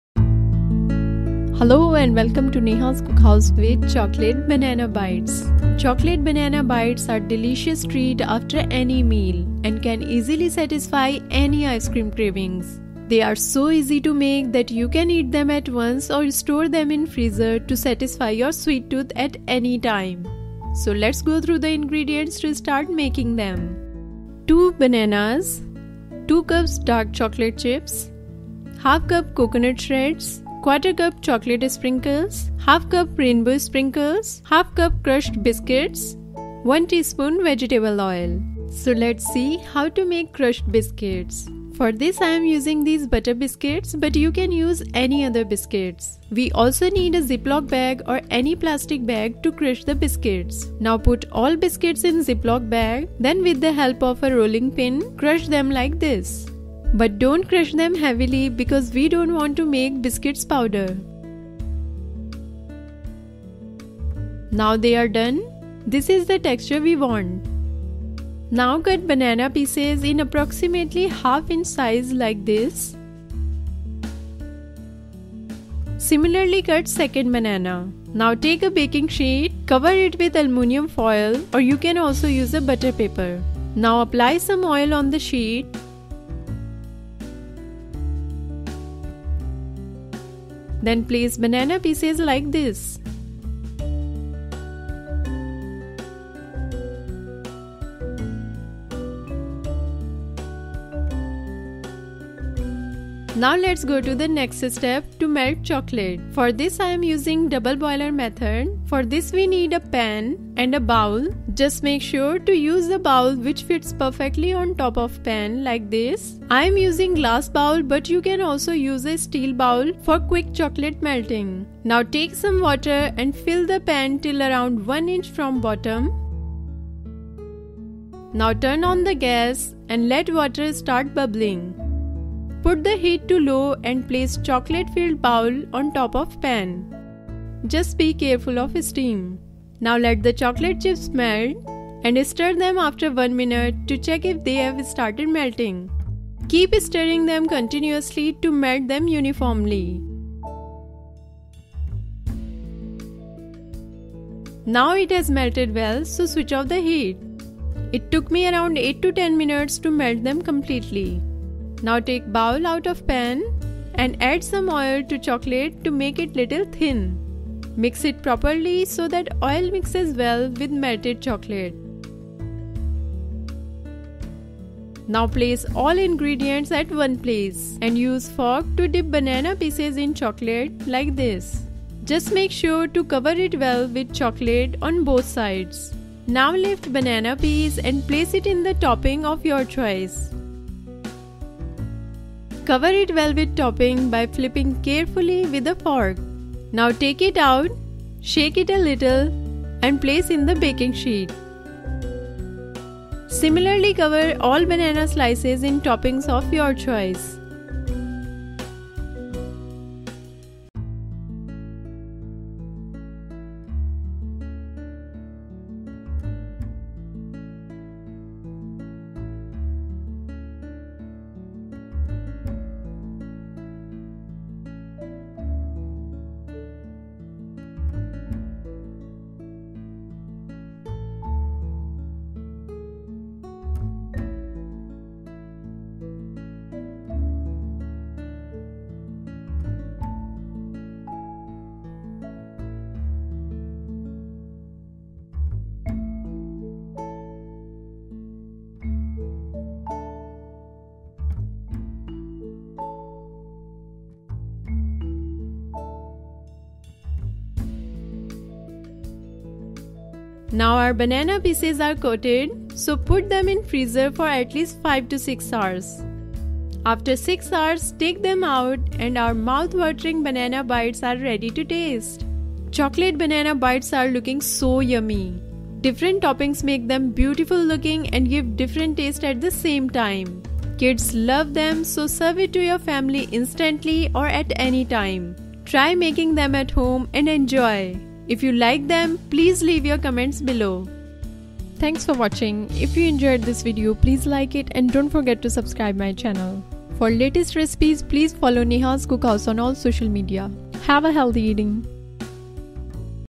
Chocolate Banana Bites Recipe | Frozen Banana Bites | How to Make Chocolate Banana Bites
Chocolate banana bites are delicious treat after any meal and can easily satisfy any ice cream cravings. They are so easy to make, you can eat them at once or store them in a freezer to satisfy your sweet tooth at any time. Different toppings make them beautiful looking and give different taste at the same time. Kids love them, so serve it to your family instantly and at any time. Try making them at home and enjoy.

Other Names:

Frozen Banana Bites

Ingredients:

2 Bananas
2 cups Dark Chocolate Chips (Sweetened)
½ cup Coconut Shreds
¼ cup Chocolate Sprinkles
½ cup Rainbow Sprinkles
½ cup crushed Biscuits
1 tsp Vegetable Oil

Instructions:

1. First lets see how to make crushed biscuits, for this I am using these butter biscuits but you can use any other biscuits. We also need a Ziploc bag or any plastic bag to crush the biscuits. Now put all biscuits in Ziploc bag, then with the help of a rolling pin crush them like this but don’t crush them heavily because we don’t want to make biscuits powder. Make sure to crush them to the texture and size shown in video.

2. Now cut banana pieces in approx ½ inch size like this, similarly cut second banana. Now take a baking sheet, cover it with aluminum foil or you can also use a butter paper. Now apply some oil on the sheet, then place banana pieces like this.

3. Now lets go to the next step to melt chocolate, for this I am using double boiler method. For this method we need a pan and a bowl. Just make sure to use a bowl which fits perfectly on top of pan like this. I am using glass bowl but you can also use a steel bowl for quick chocolate melting. Now take some water and fill the pan till around 1 inch from bottom. Now turn on the gas and let water start bubbling. When it starts bubbling put the heat to low and place chocolate filled bowl on top of pan, just be careful of steam. Now let the chocolate chips melt and stir them after 1 min to check if they have started melting. Keep stirring them continuously to melt them uniformly. Now it has melted well so switch off the heat. It took me 8-10 mins to melt them completely.

4. Now take the bowl out of pan and add some oil to chocolate to make it little thin. Mix it properly so that oil mixes well with melted chocolate.

5. Now place all ingredients at one place, and use a fork to dip banana pieces in chocolate like this. Just make sure to cover it well with chocolate on both sides. Now lift banana piece and place it in the topping of your choice, cover it well with topping by flipping carefully with a fork. Now take it out shake it a little and place in the baking sheet. Similarly, cover all banana slices in toppings of your choice.

6. Now our banana pieces are coated, so put them in a freezer for atleast 5-6 hrs. After 6 hrs take them out and our mouth watering Banana bites are ready to taste.

Tips n Tricks:

1. Make sure your bananas are not too soft as they will break while dipping in chocolate.
2. You can use any toppings of your choice.